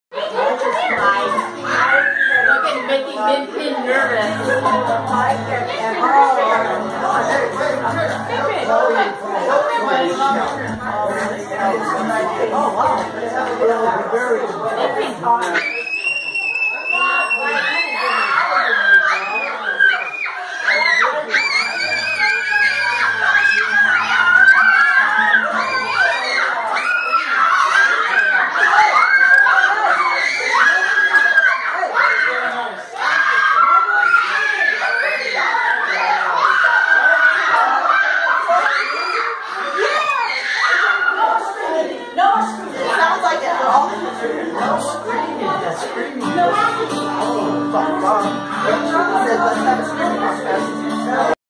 Perez Thankksgiving 2015
Fun @Rosie 'n Al's place! Maxton plays guitar! Kids scream!